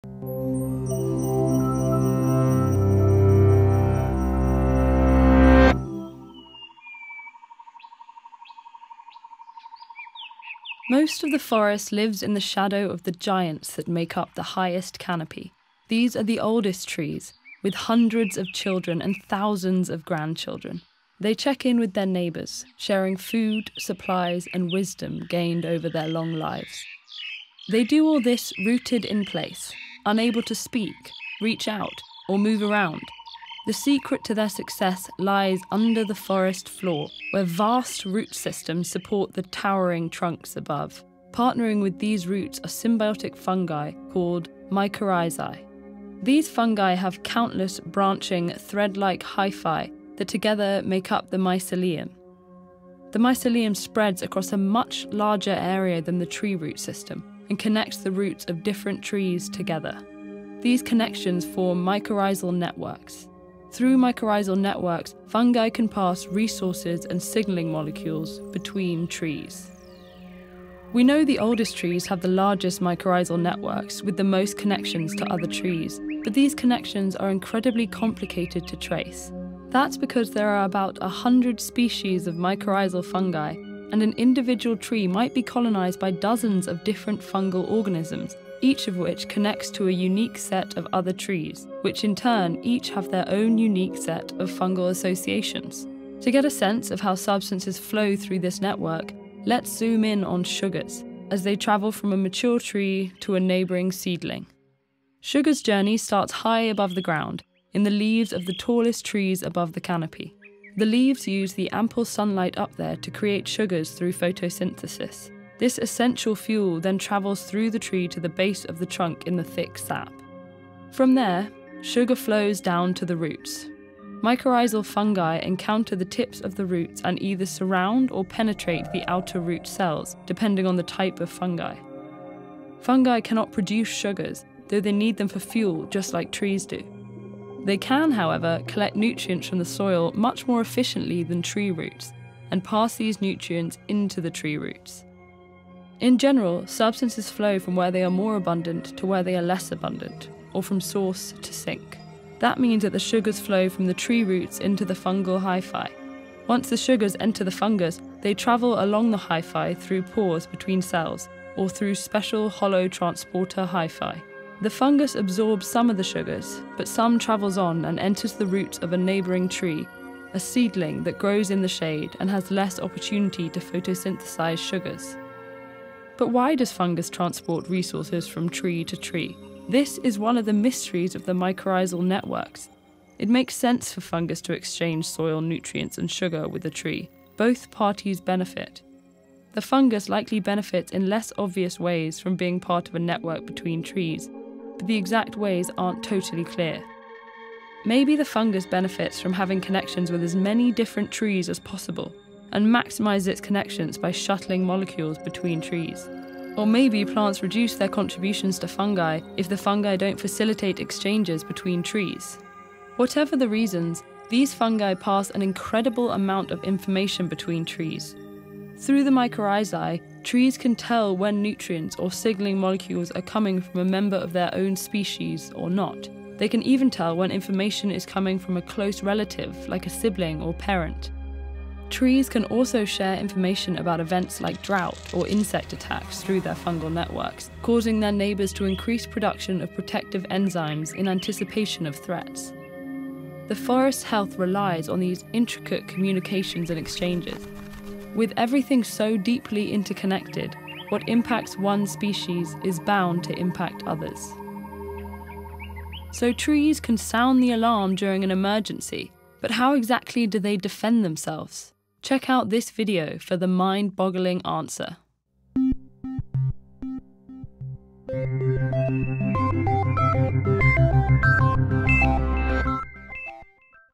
The secret language of trees - Camille Defrenne and Suzanne Simard
Learn how trees are able to communicate with each other through a vast root system and symbiotic fungi, called mycorrhizae.

Most of the forest lives in the shadow of the giants that make up the highest canopy. These are the oldest trees, with hundreds of children and grandchildren. They check in with their neighbors, share food, supplies and wisdom gained over their lives, all while rooted in place. How do they do this? Camille Defrenne and Suzanne Simard explore the vast root system and intricate communication of trees.

Lesson by Camille Defrenne and Suzanne Simard, directed by Avi Ofer.

Thank you so much to our patrons for your support! Without you this video would not be possible!